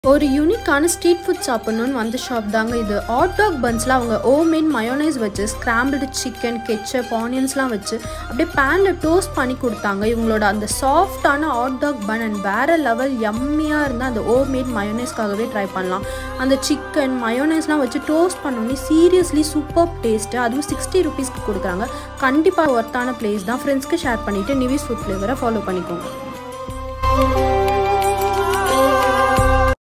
Chicken hot dog bun🌭 | Pondy food | Nive’s Vlog
Chicken hotdog bun🤤❤️Send to ur frnz and try this💯

📍 - FE fast food near Pondy railway station ( Exact opposite to St. Basilica church) , Pondicherry.
💰 - ₹60 ( Chicken Hotdog Bun)

⏱-Will be opened at evening to night oly!

Nive's Review 📝

This chicken hotedog Bun we have tried recently seriously the taste was tooo good its a simple road side shop near church, Firstly that bun was tooo soft to wat and yummm and the mayonnaise which they do by themself looked so eat when we consume also it is vera level you can try this they also had beef hotsog and sandwiches etc!

Follow @nivesfoodflavour for more yummy foods!!!

NO REPOST❌